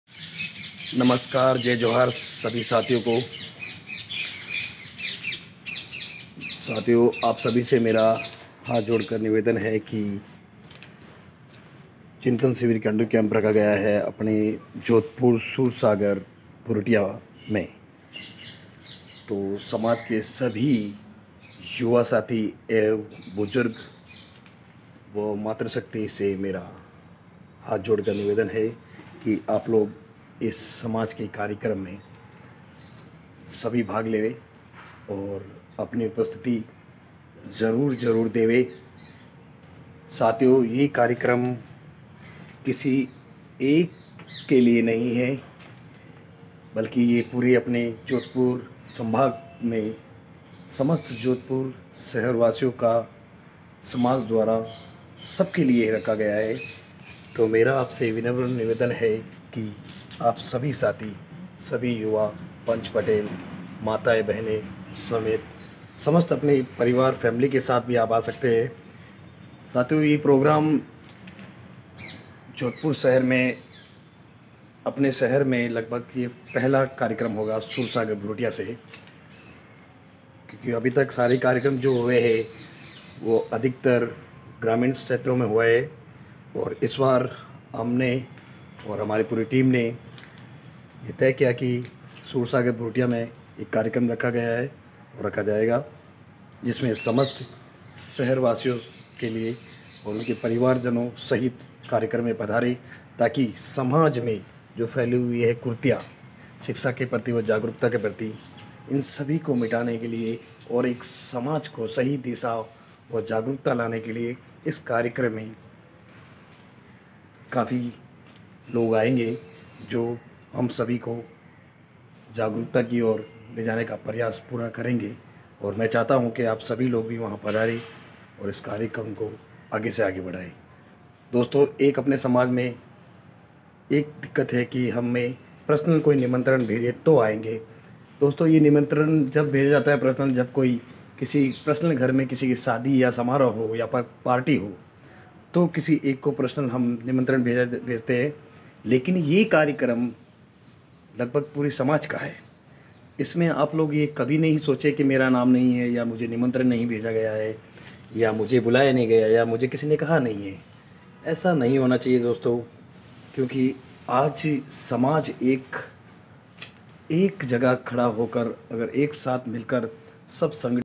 भील आदिवासी परिवार चिंतन 23.12.2022सुबह 10:आओ हम सब मिलकर समाज को एक नई दिशा or शिक्षा की ओर ले चले
परिवार चिंतन शिविर केड़र

केम्प दिनांक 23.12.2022 वार शुक्रवार समय सुबह 10:00 .आओ हम सब मिलकर समाज को एक नई दिशा or शिक्षा की ओर ले चले .